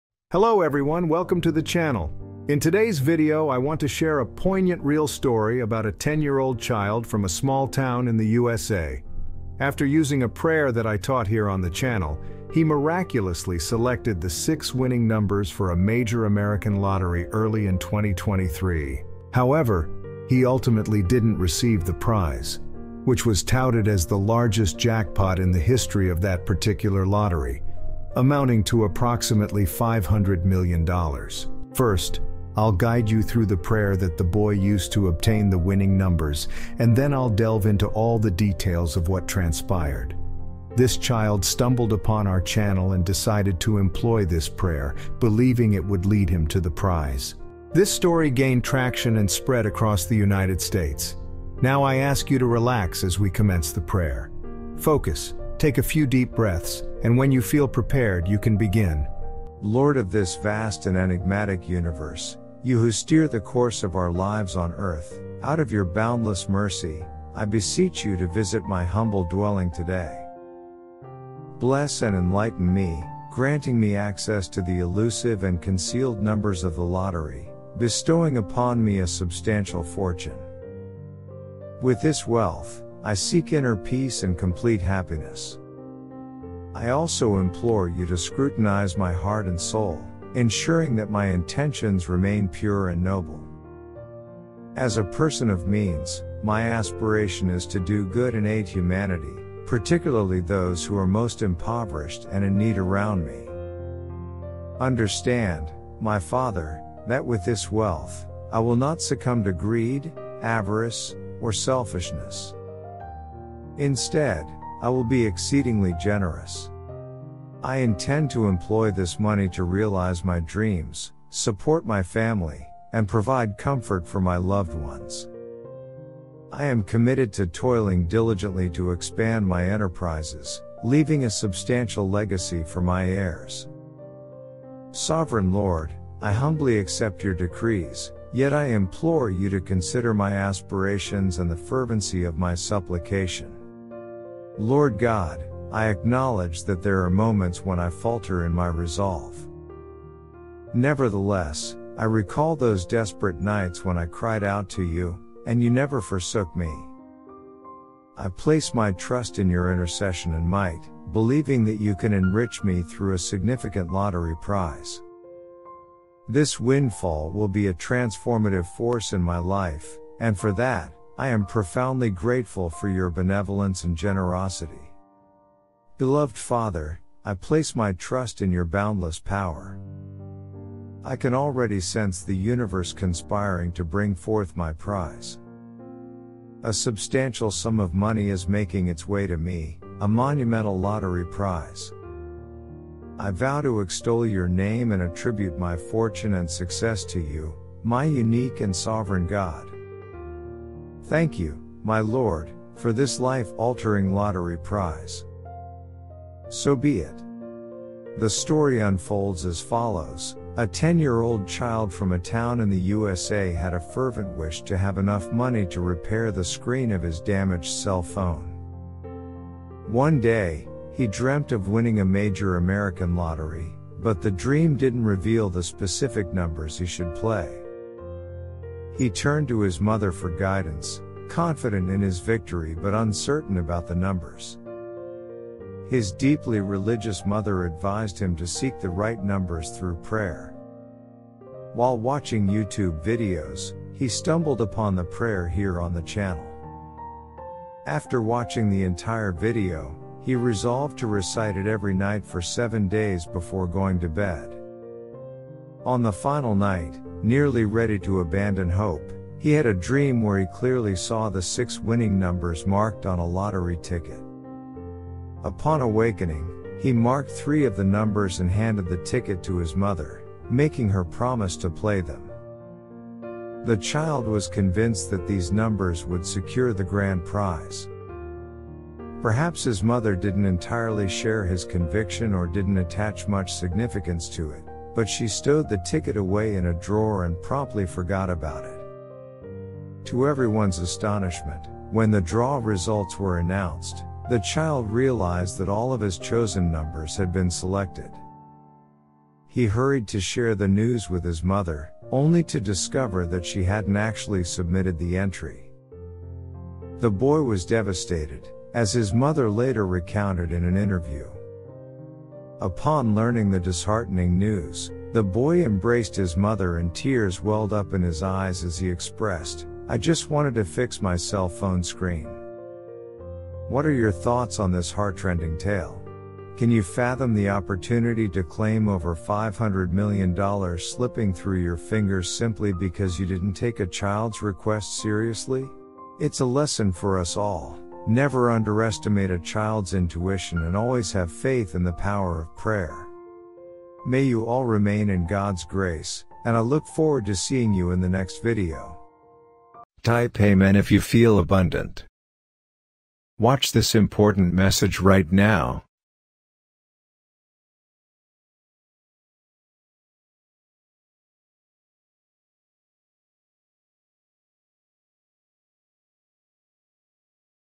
BOY MATCHES ALL SIX NUMBERS IN THE MEGA MILLIONS BUT HIS MOTHER DIDN'T PLAY THE TICKET
Imagine the shock of matching all six numbers in the Mega Millions, only to realize the ticket was never actually played! In this emotional rollercoaster of a story, a young boy picks the winning numbers, but due to a twist of fate, his mother didn't purchase the ticket. 😱

Join us as we recount this unbelievable story—what could have been a life-changing moment turned into one of the biggest “what-ifs.” Hear firsthand reactions from the family, and learn how they dealt with the heartbreak of missing out on a massive fortune.